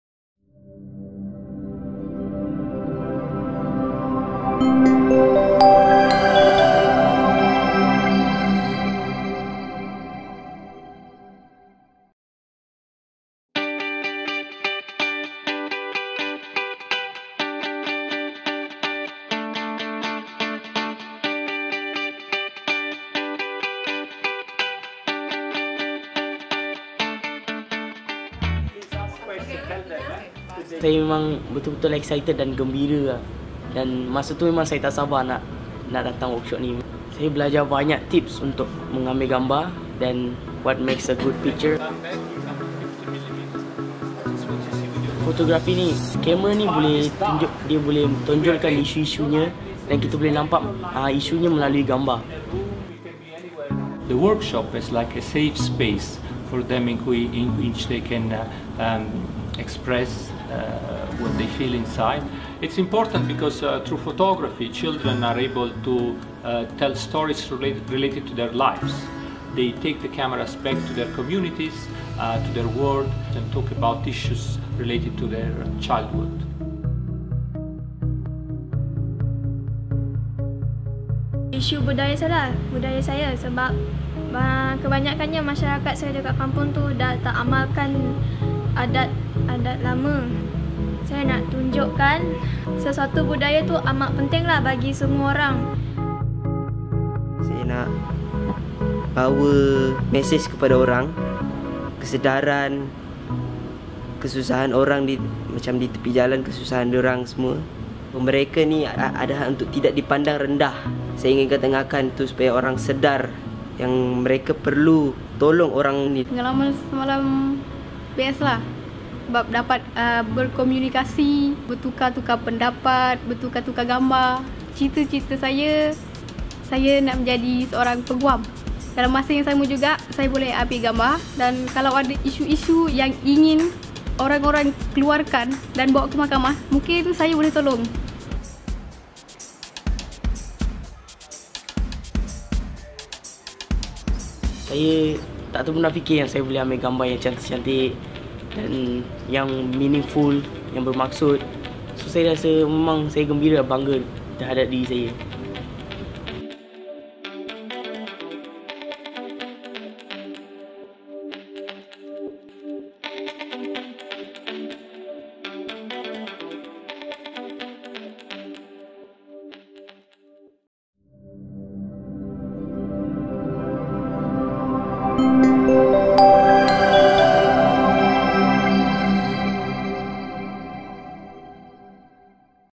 The World We Want Teen Photo Workshop
This UNICEF Malaysia video brings together young Malaysians, refugees and youth from the country's marginalized indigenous communities for a workshop by humanitarian photographer, Giacomo Pirozzi, on how to use photography to tell stories about their lives. The program was held in conjunction with the 25th anniversary of the Convention on the Rights of the Child.